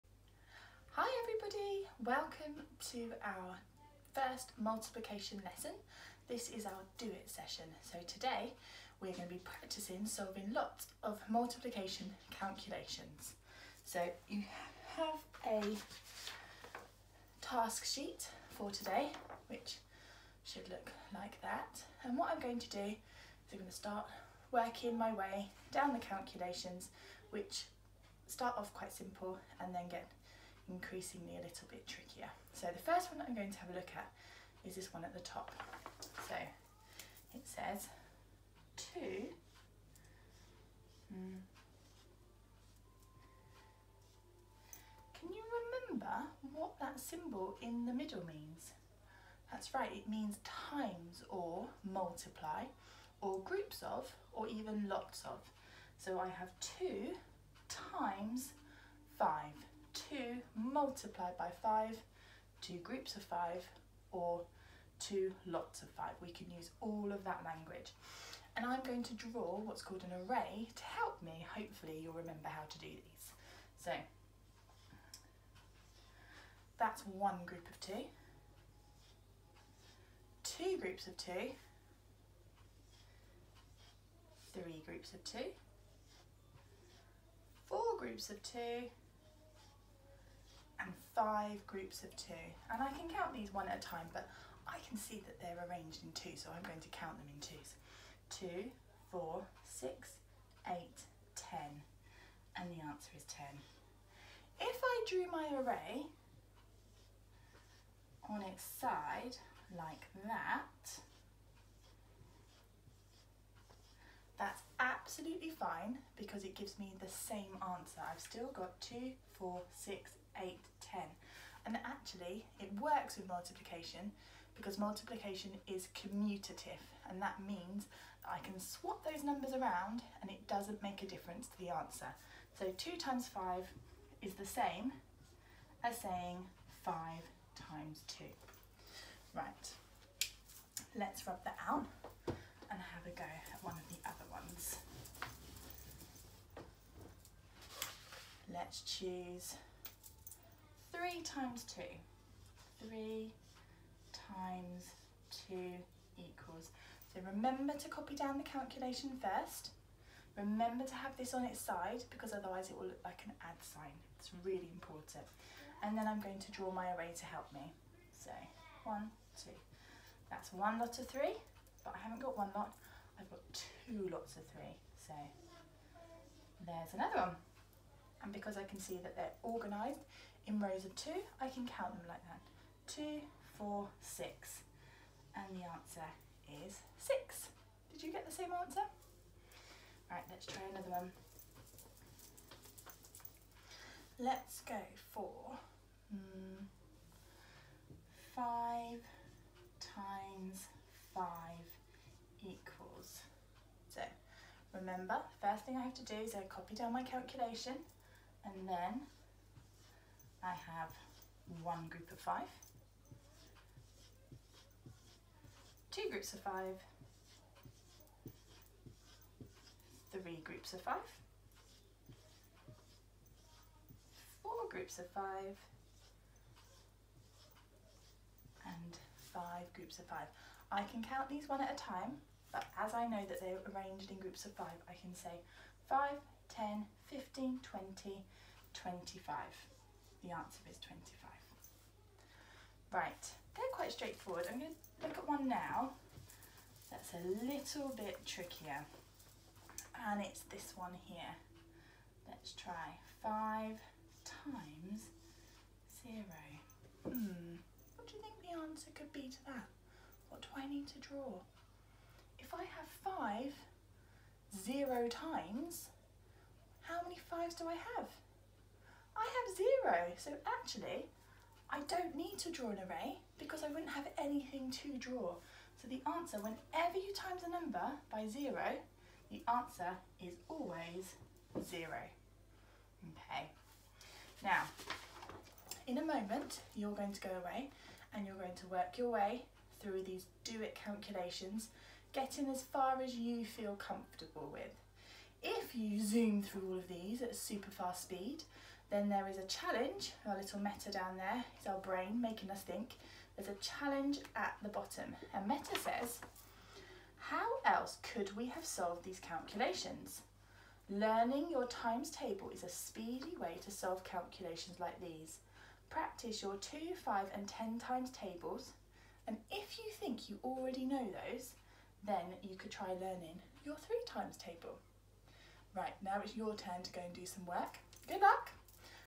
Year 2: Maths - Multiplication DO IT
In this video Miss McDonagh shows you how to complete the Multiplication DO IT activity.